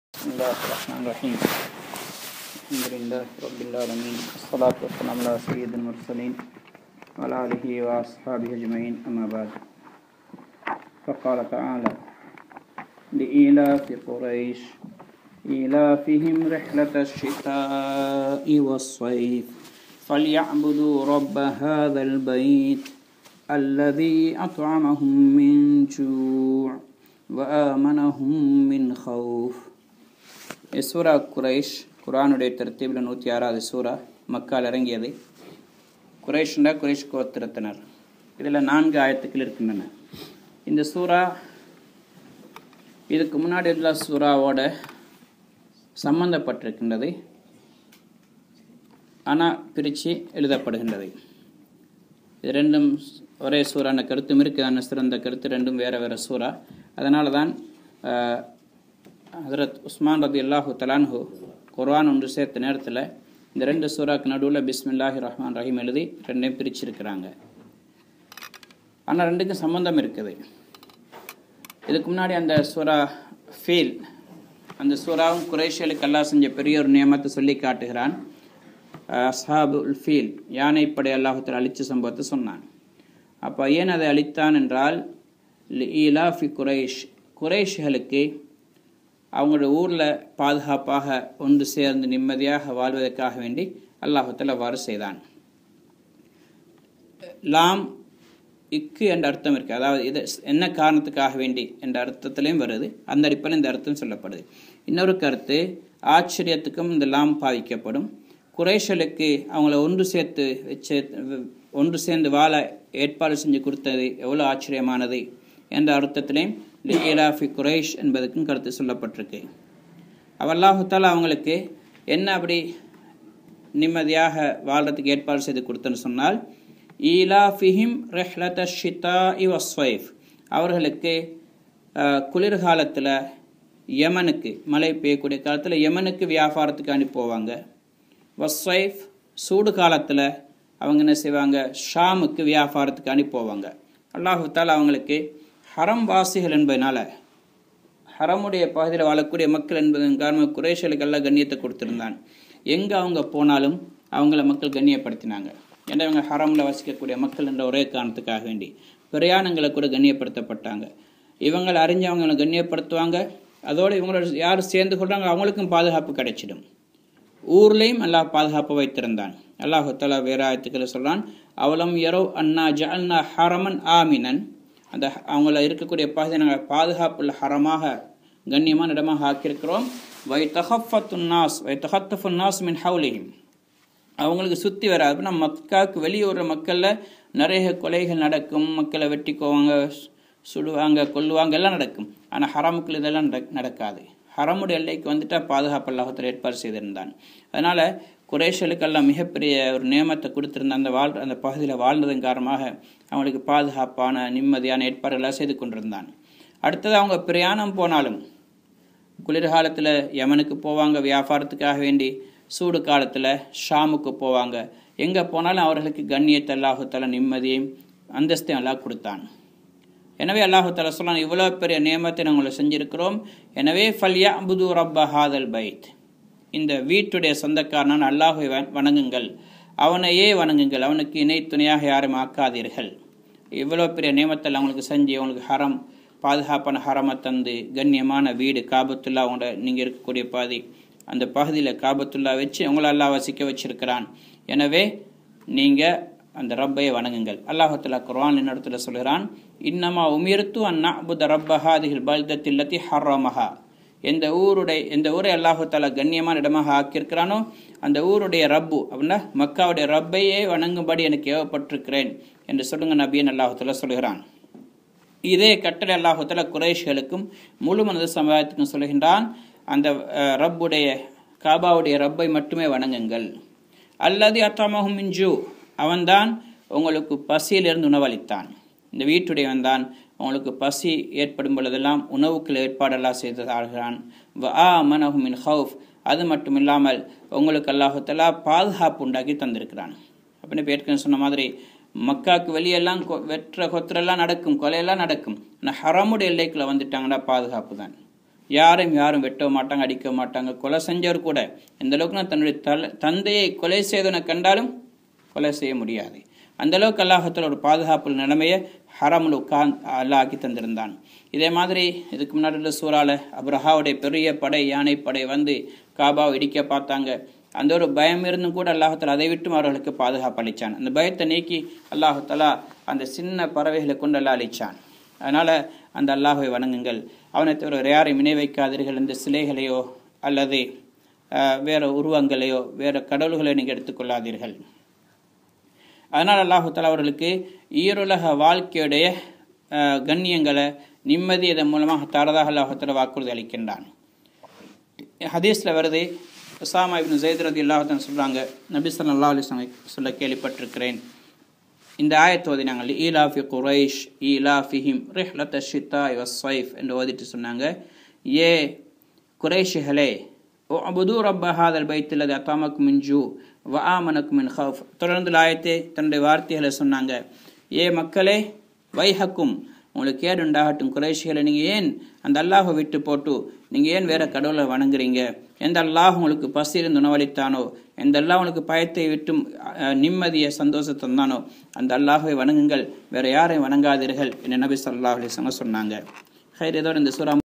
Ash Sheikh Anas Mufti (Deobandi) Rawlathut Tafseer -291-Surah Qureish in Tamil
Tafseer of Quran @ Shonandai, Japan By Mufti Anas in Tamil on Surah Qureish 01-04 Ayats